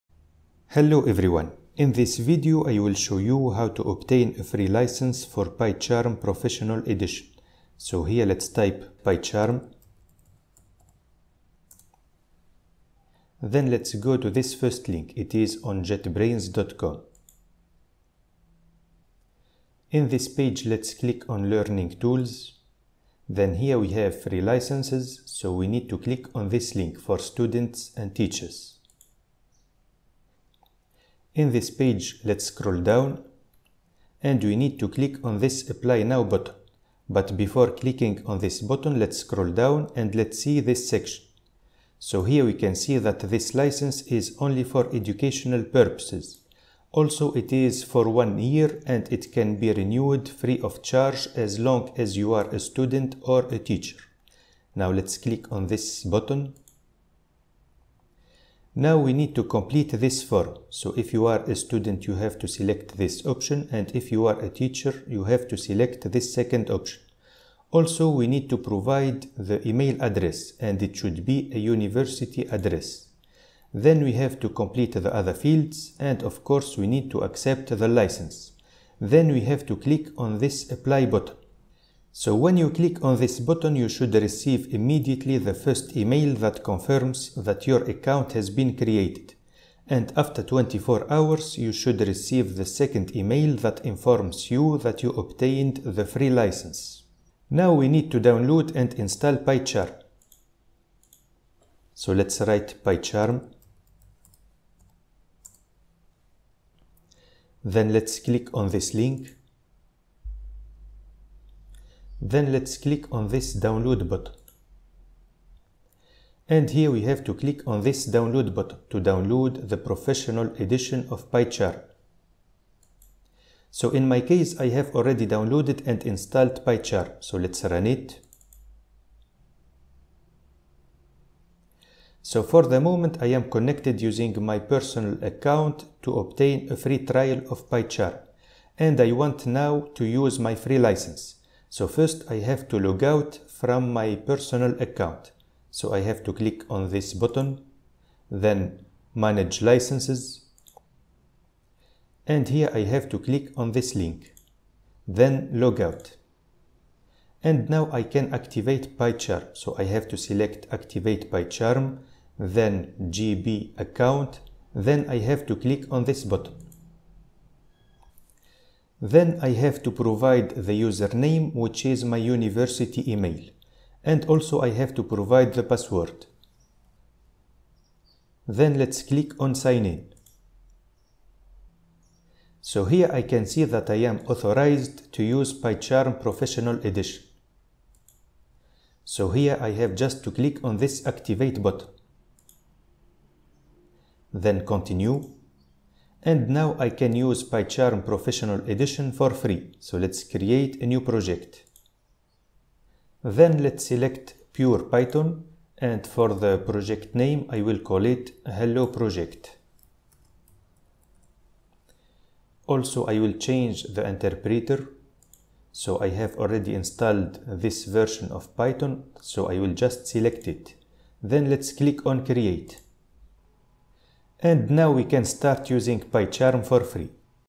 Get a Free License for PyCharm Professional and all JetBrains Products for Students and Teachers
How to install and use PyCharm Professional 2022 for free. How to get a free license for PyCharm Professional, IntelliJ Ultimate, CLion, WebStorm, PhpStorm and others. How to switch between JetBrains accounts. How to activate JetBrains license for education. .